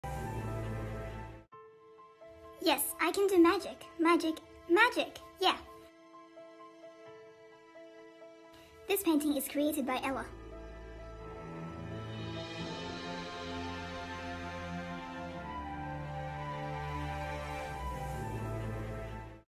Animated Story - Ella‘s Magic
Come and join Starry Arts online painting + animation classes! We bringing children’s art to life!
Take a look at this animated story, 'Ella's Magic' created by our student,  Ella Wu from their original artwork!